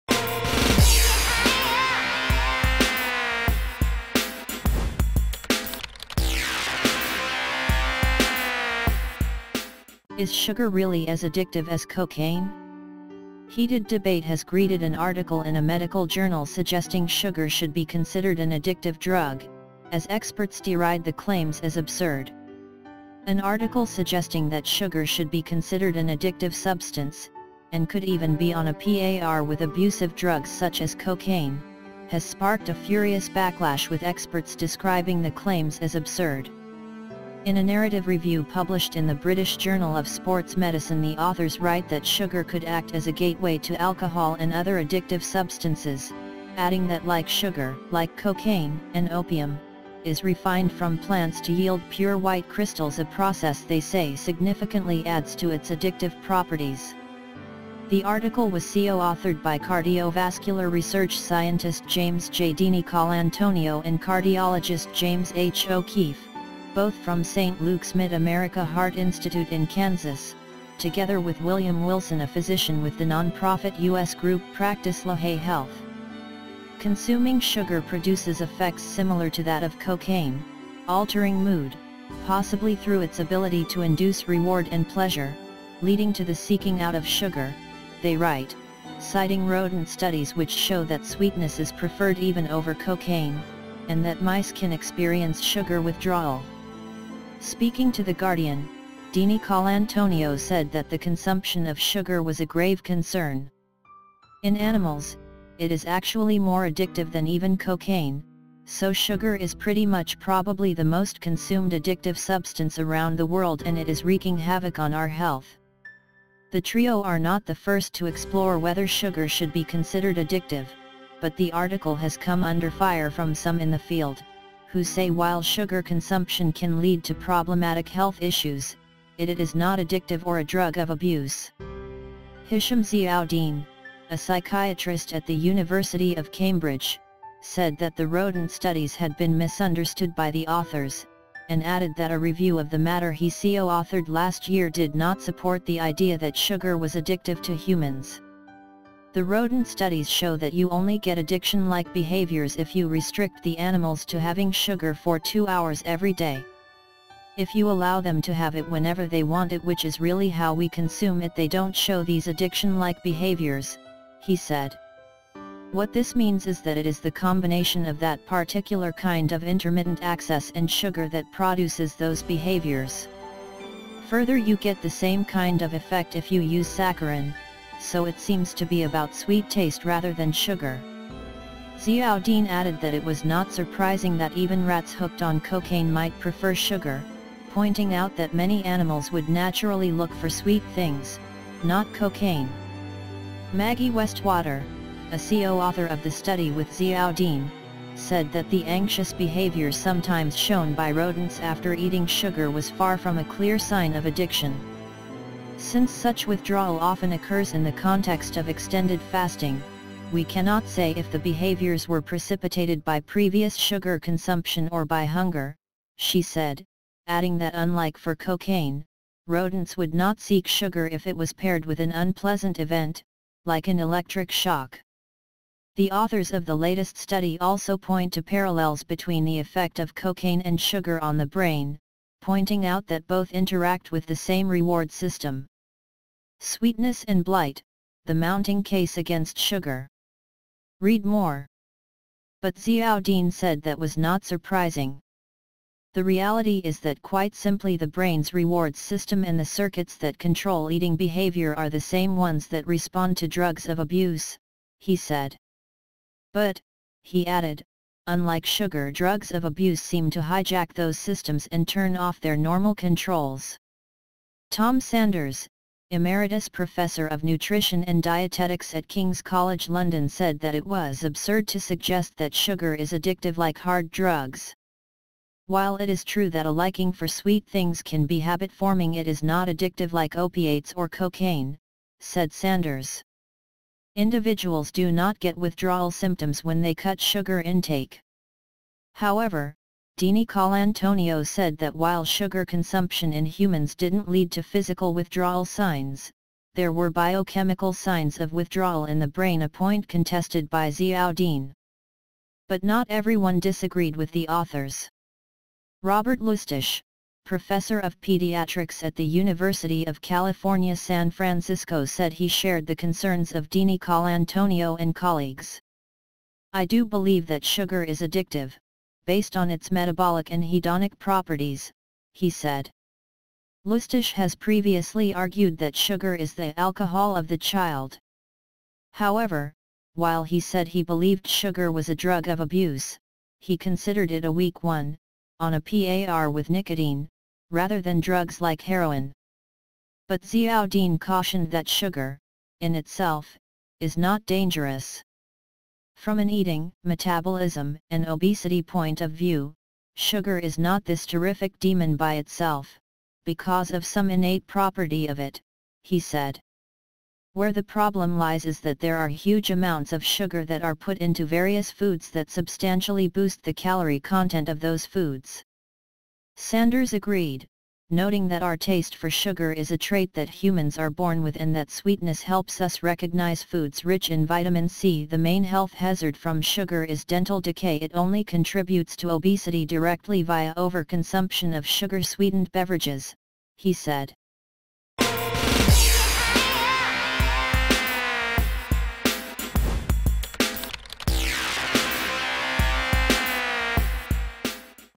Is sugar really as addictive as cocaine
Heated debate has greeted an article in a medical journal suggesting sugar should be considered an addictive drug, as experts deride the claims as ‘absurd’